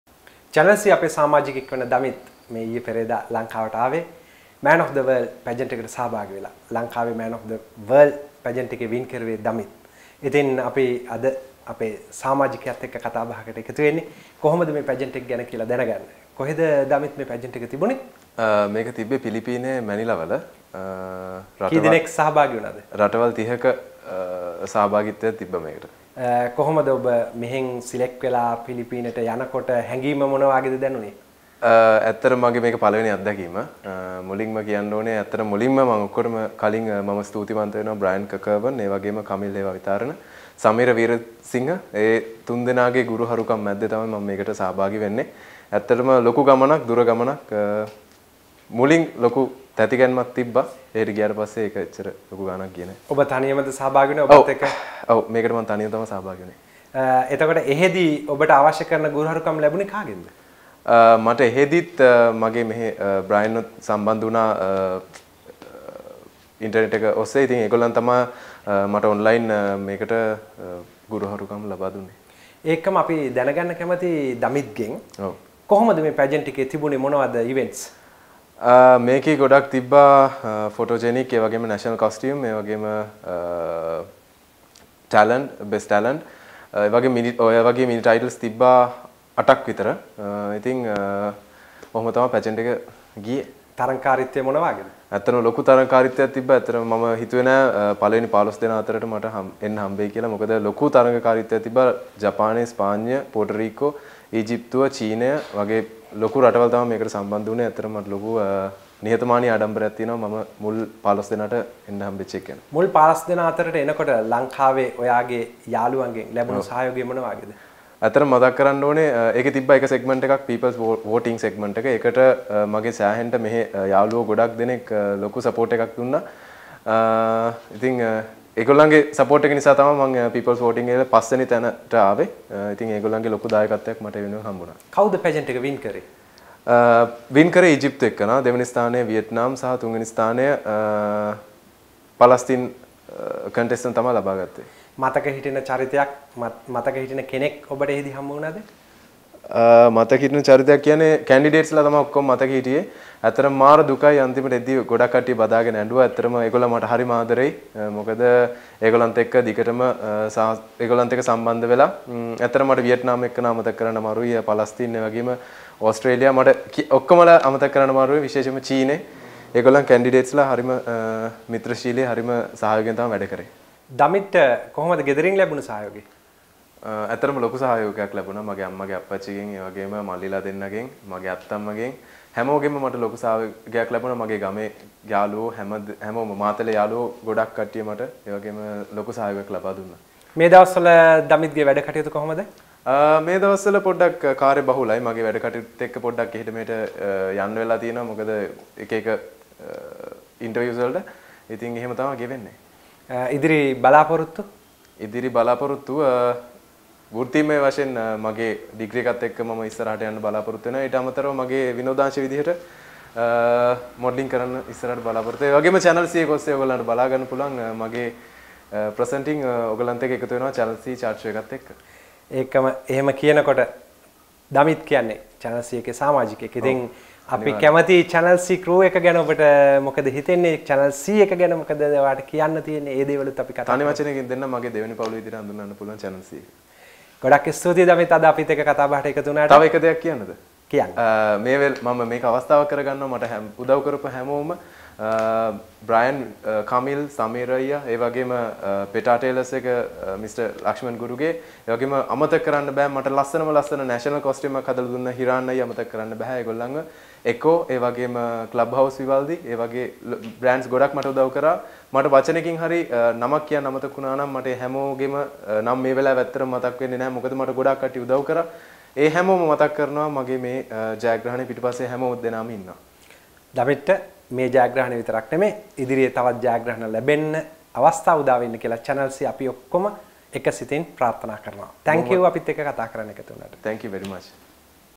Srilankan Top Model Damith Wijethunga is participated to the pageant of Man Of The World !
Man Of The World ශ්‍රි ලංකාවේ  නියෝජනය දමිත්  විජේතුංග